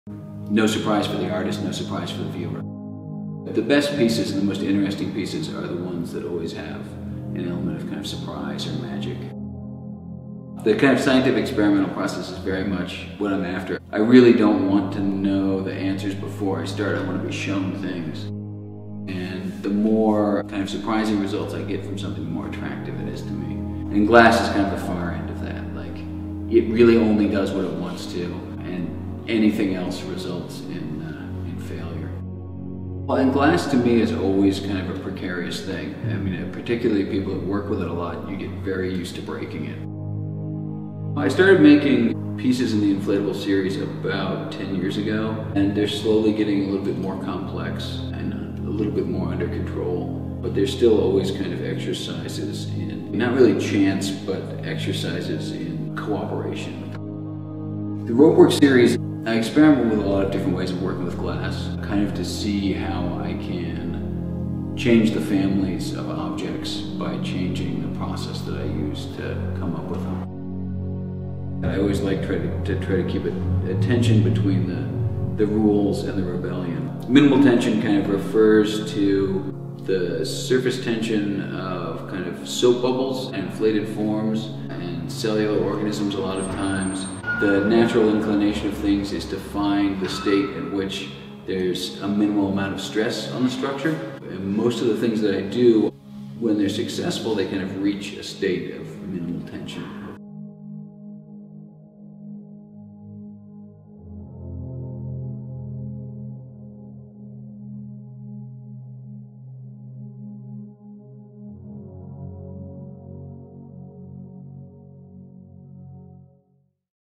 Interview: Matthew Szösz, Glass Artist
Award-winning glass artist Matthew Szösz debuts new works from two of his ongoing series, Inflatables and Ropework, in his solo exhibition Minimal Tension.

For Szösz, setting up just one glass art experiment is an involved process. The preparation takes half a workday in some cases, and up to four weeks in others. It all leads to that pivotal moment, when the sculpture either takes its final shape or shatters into a million pieces. The Seattle-based artist has repeated this process countless times—with about 75% of his work instantly collapsing into rubble. This spirit of fearless experimentation is reflected in his dazzlingly innovative, award-winning oeuvre.